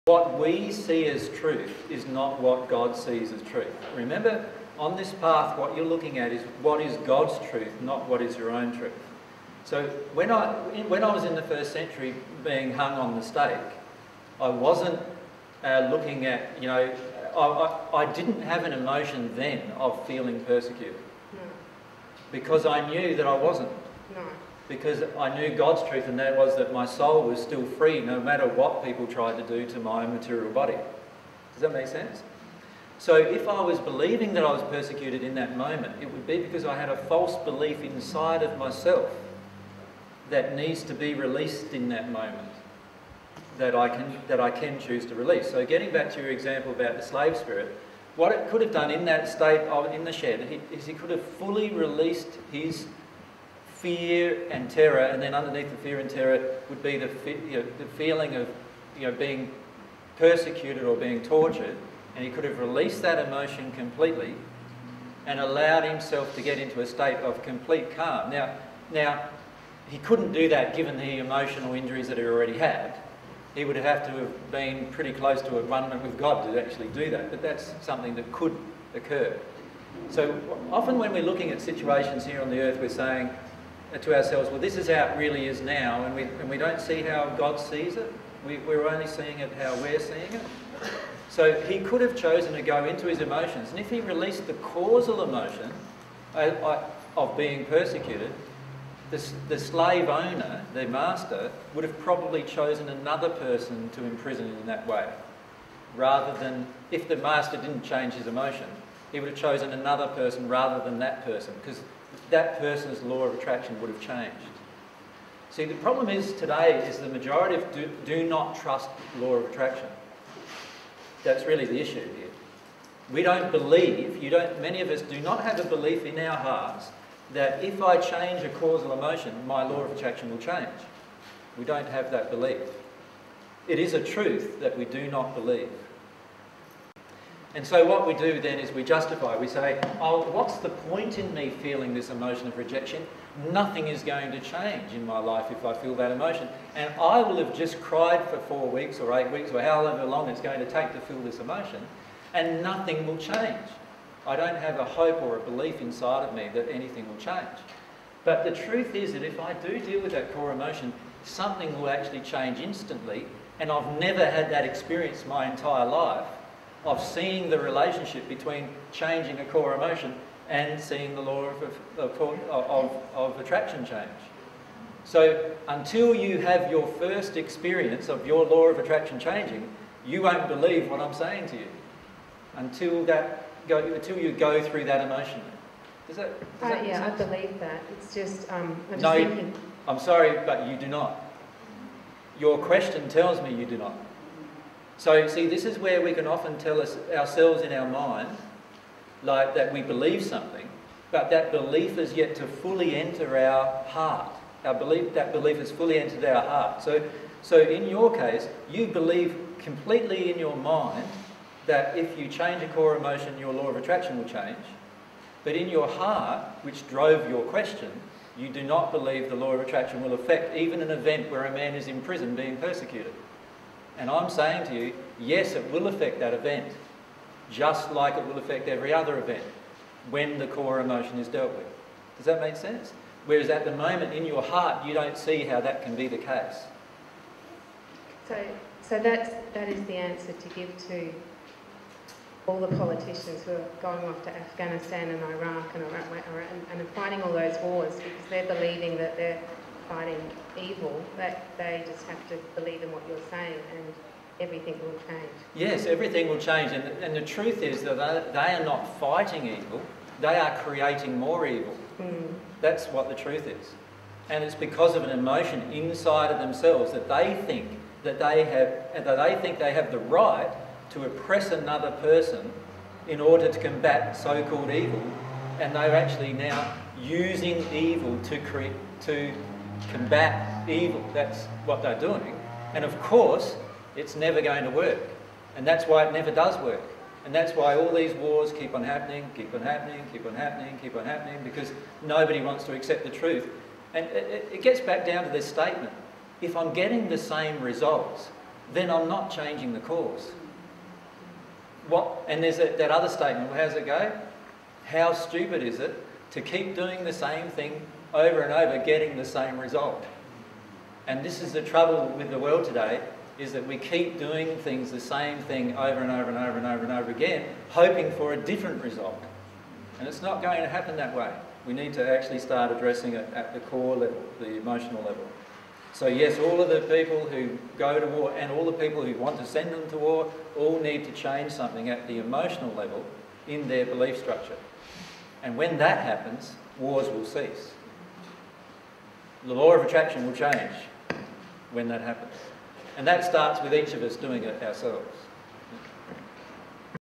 Jesus on How Oppression and War Will Cease on Earth
Jesus explains that as we work through false beliefs and release the pain within our own souls, our attractions and our world will change for the better.

This is a section of the presentation by Jesus and Mary entitled "20090426 The Human Soul - Anger Is Your Guide S2".

dtstart:004310:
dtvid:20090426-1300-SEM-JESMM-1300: